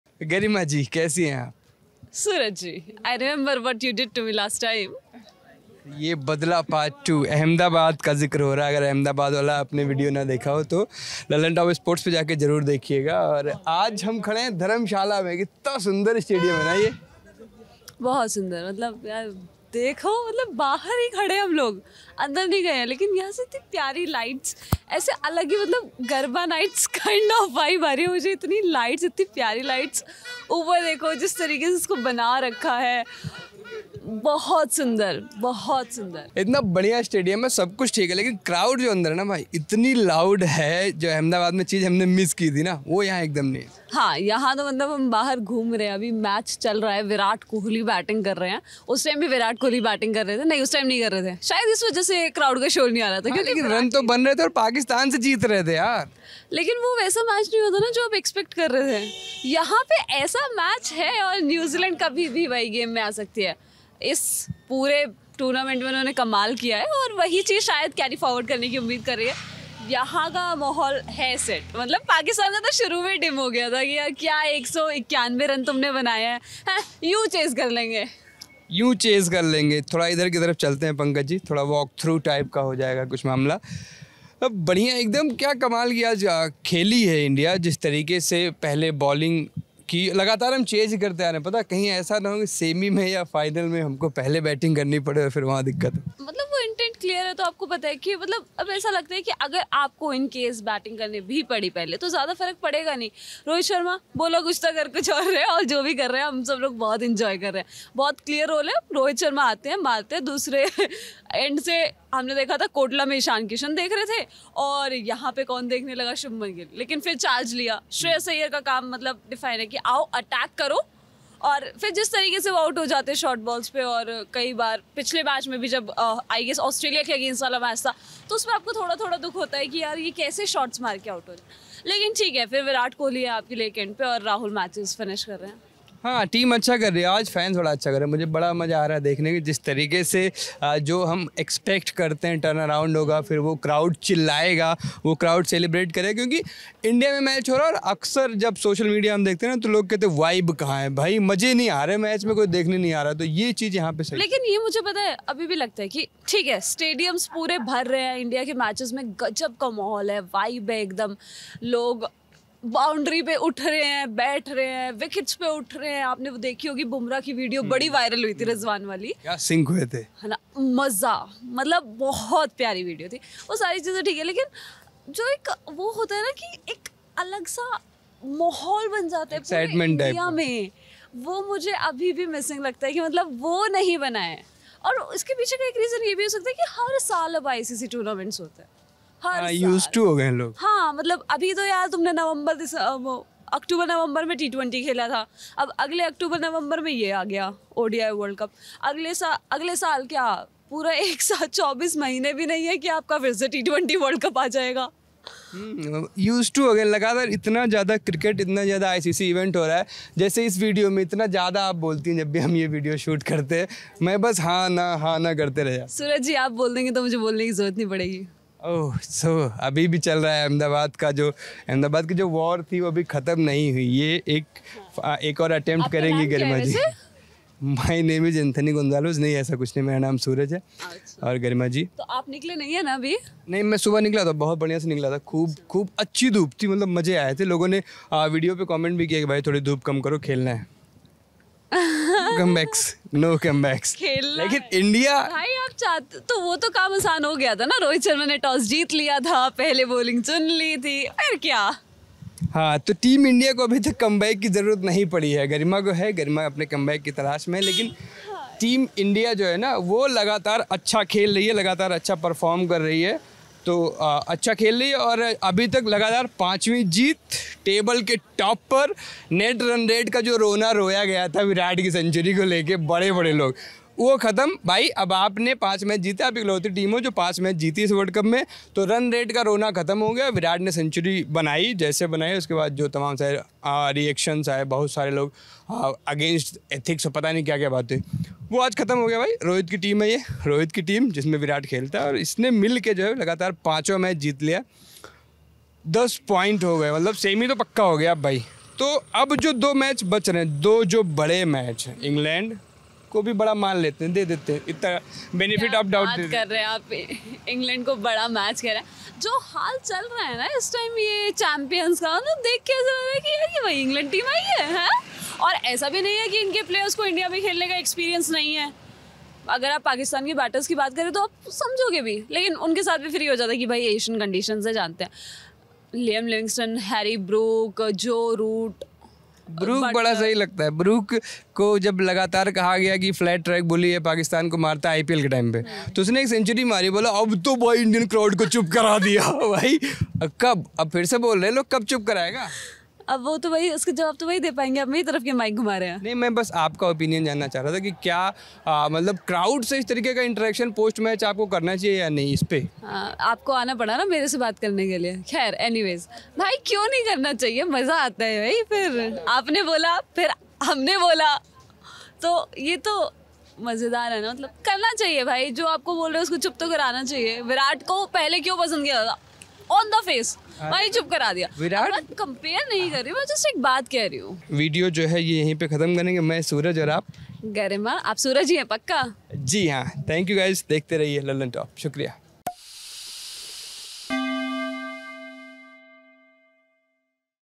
Virat Selfish Batting, हंगामा करने वाले अब शांत हो जाएंगे? India vs Pakistan Ahmedabad | WorldCup
India vs New Zealand. Team India has finally beaten NZ in a big match. After Mohd Shami, Kuldeep Yadav, Jasprit Bumrah, Mohd Siraj and Ravindra Jadeja's bowling effort, Rohit Sharma Virat Kohli did well in the batting. Captain Rohit Sharma gave Team India the much needed start, Virat carried it forward and scored 95. In between all this, fans too kept cheering for the team, this sound can be heard outside the stadium. Watch and try to feel the vibe of Dharmshala HPCA stadium through the video.